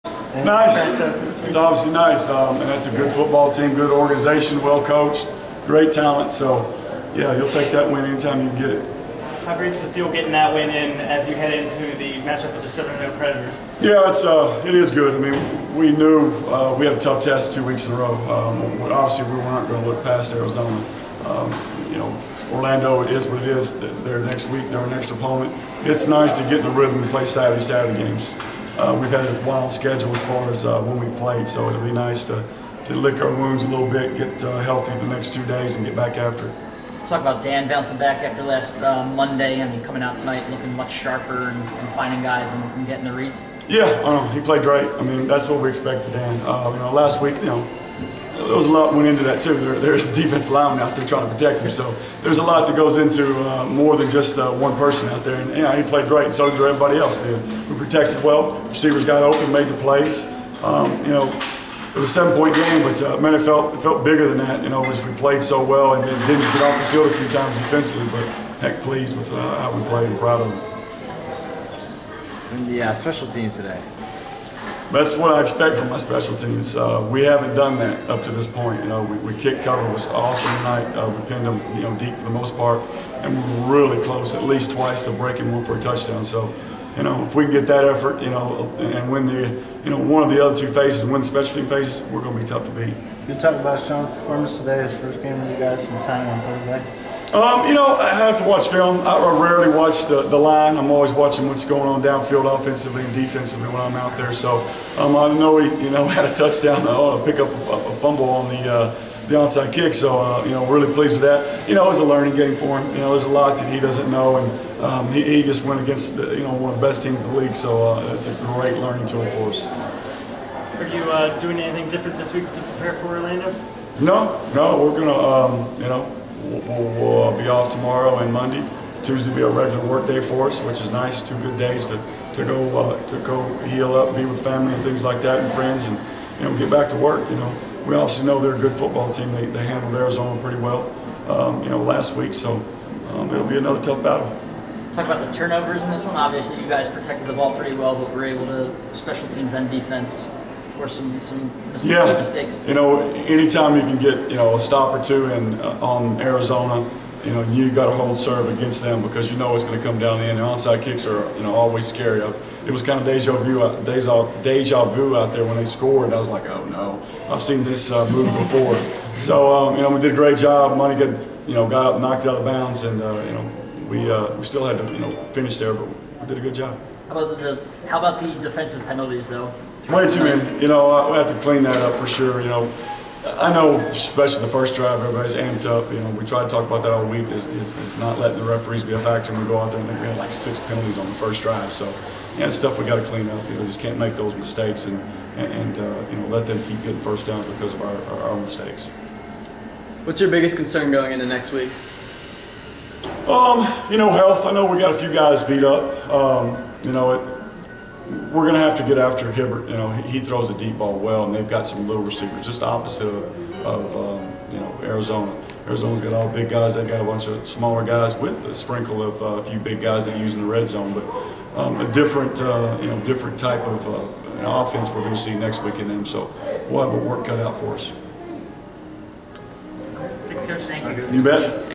Philadelphia Soul's Head Coach Clint Dolezel Post-Game 5-21-16
Philadelphia Soul's Head Coach Clint Dolezel, discusses the team's 65-58 victory over the Arizona Rattlers.